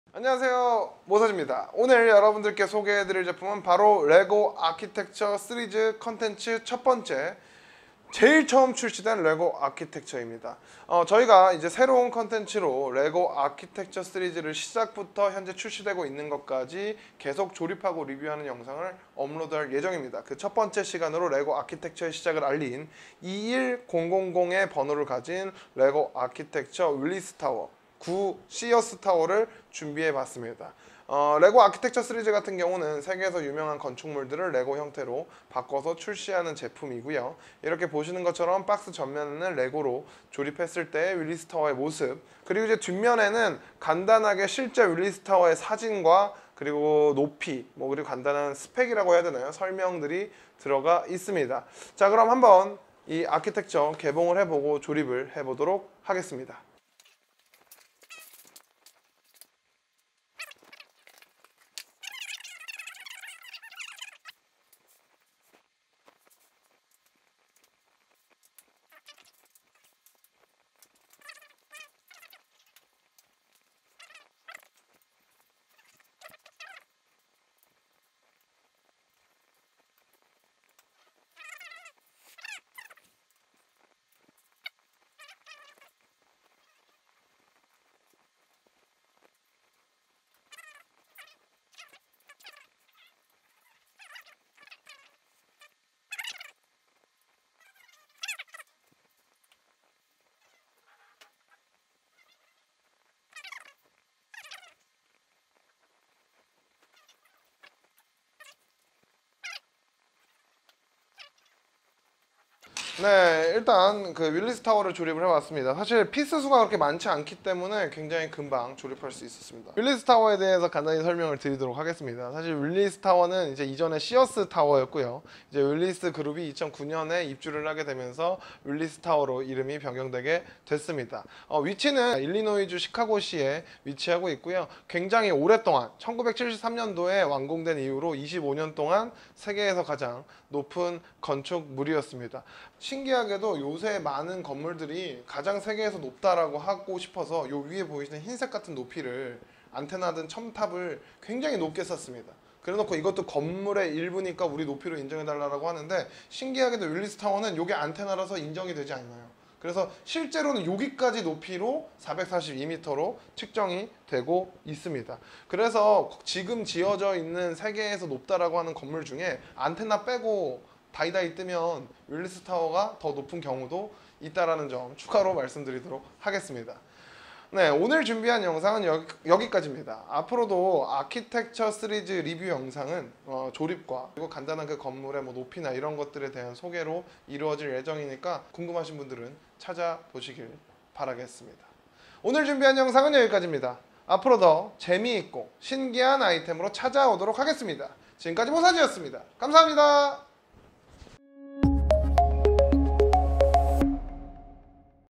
[오늘 뭐 사지?] 레고의 첫 아키텍쳐 21000 윌리스 타워 (LEGO Architecture 21000 Willis Tower Unboxing)
새로운 컨텐츠. 레고 아키텍쳐 컨텐츠의 첫 시작은 레고에서 처음으로 출시한 첫 아키텍쳐인 21000 윌리스 타워로 시작합니다.

윌리스 타워는 국내에서 시어스 타워로 알려져 있으며 2009년 이름이 윌리스 타워로 변경되었습니다. 1973년부터 25년간 세계에서 가장 높은 건축물로 이름을 알렸습니다.

앞으로 레고에서 출시한 아키텍쳐 시리즈를 전종 리뷰할 계획이며 21000 부터 현재 출시 되는 모든 제품까지 단종된 제품도 모두 리뷰 할 예정입니다. (사실 레고 아키텍쳐는 다 가지고 있습니다 후후.)

궁금한 점은 댓글 남겨 주시면 답변 드리도록 하겠습니다.

가격 : 19.99불 (현재 단종되었습니다.)

장점
 - 아키텍쳐 시리즈는 멋있습니다.
 - 그리고 조립이 빨리 끝나죠.
 - 인스트럭션에 포함되어 있는 제품 설명이 아주 아주 좋습니다.

단점
 - 정가 19.99 불인데도 뭔가 아쉬운 느낌의 피스수.

"레고 아키텍쳐 시리즈. 반응이 좋으면 아키텍쳐가 이벤트로 찾아올 수도?"

Mosazi Instagram : 사진으로 보다 빠르게 만나실 수 있습니다.